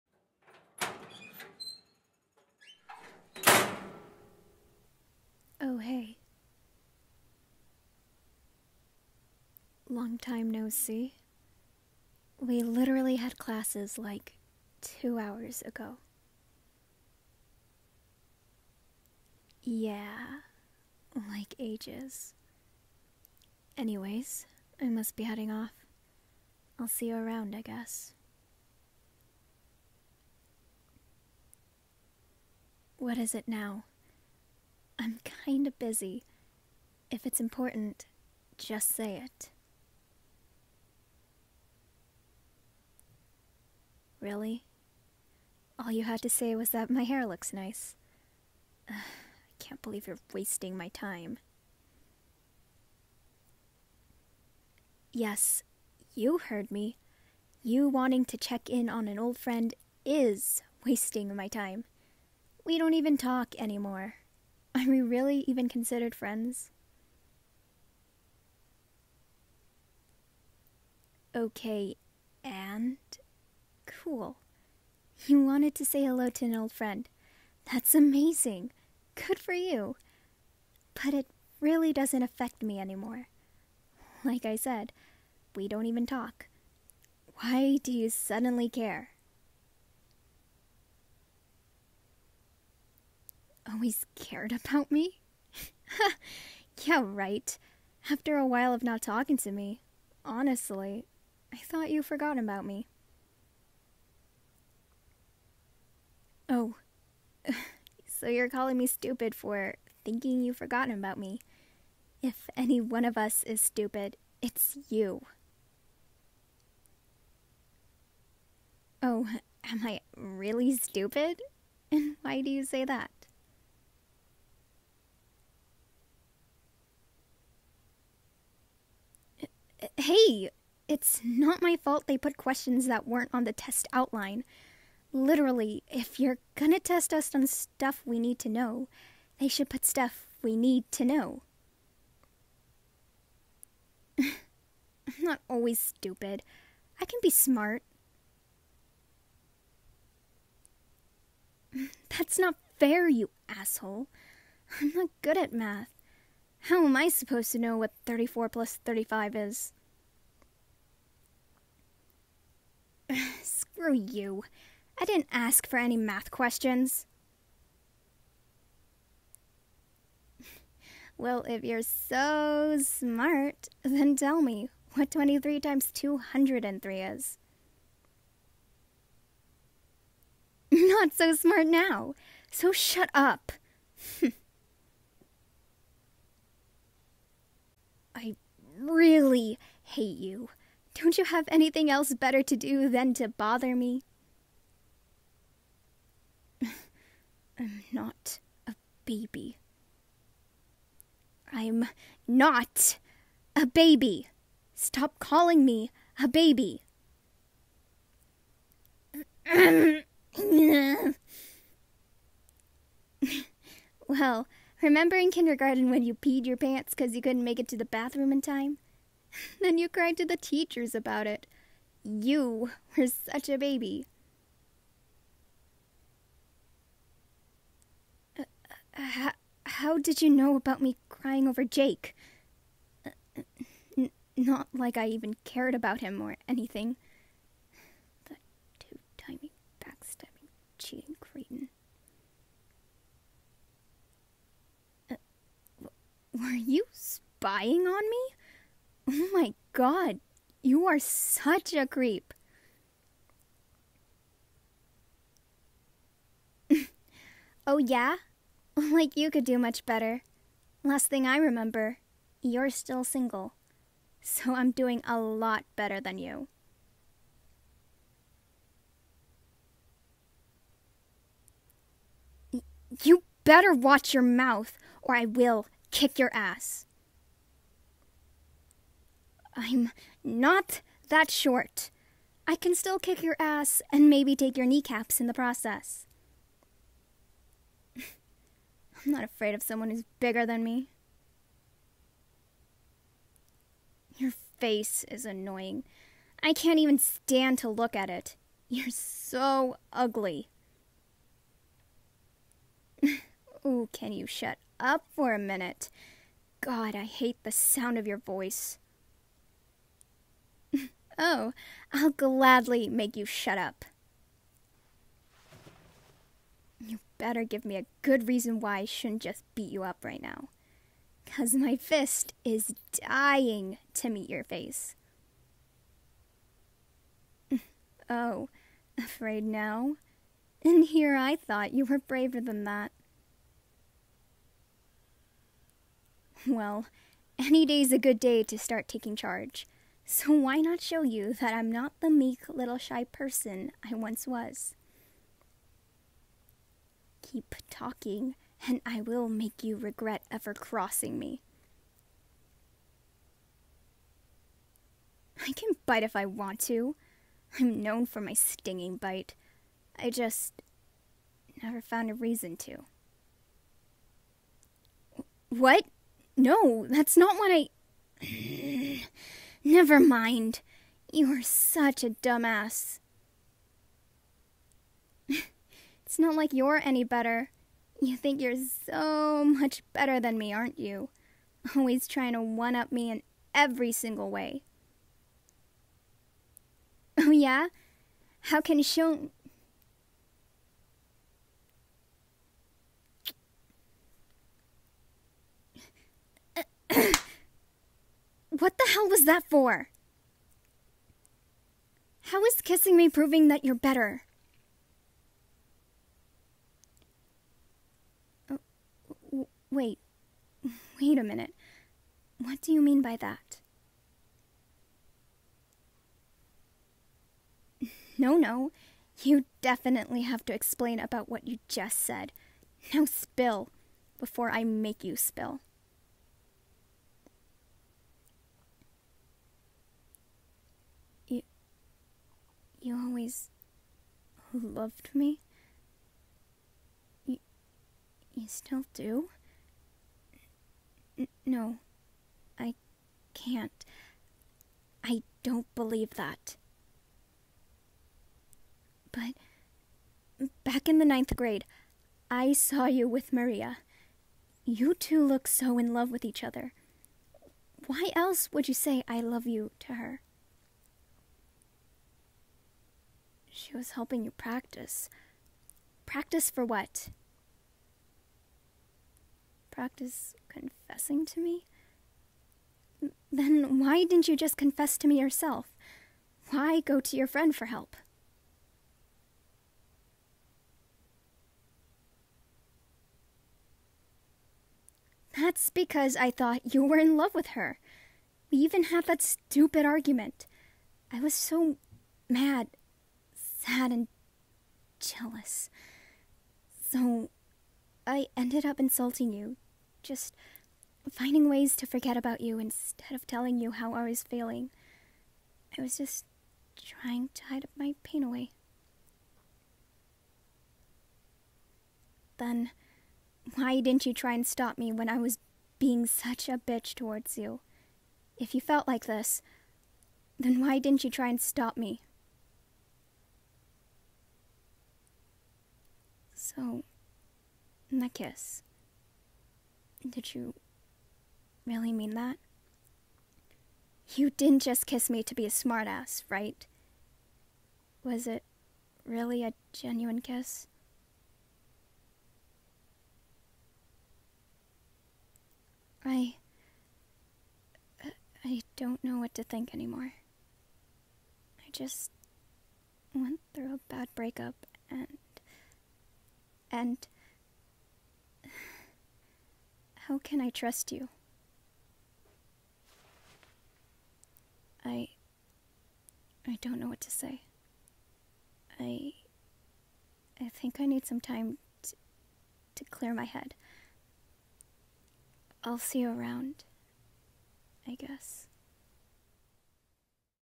My Heartless Sweetheart - Audio RP [F4A] [Enemies to Lovers] [Confession]
It’s been a few years since your falling out with your best/childhood friend, no matter what you did, she would always cut you off before you could get through to her.

You’d never let her win though, you were always one to never back down from a fight but your feelings for her only grew. She was almost no longer the meek girl you once knew.

When you accidentally slip up and confess, that’s when your true feelings about her rise to the surface.

*:･ﾟ✧*:･ﾟ✧

Script was made by me :3
Script Idea by Fourze

First part of the "My Heartless Sweetheart" series

*:･ﾟ✧*:･ﾟ✧

Thumbnail art made by me :3

*:･ﾟ✧*:･ﾟ✧

Socials